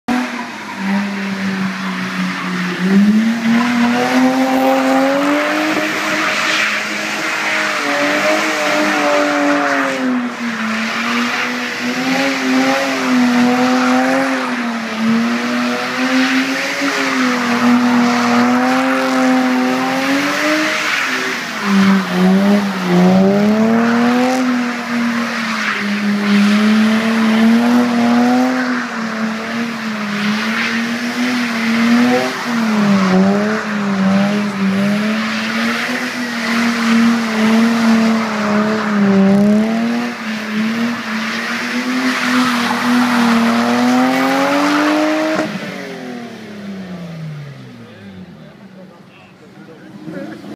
romanegila Edmonton. Stacey av. Every sat&san BMW e30 325i
Drifting place Stacey av. Edmonton sat&san 7 pm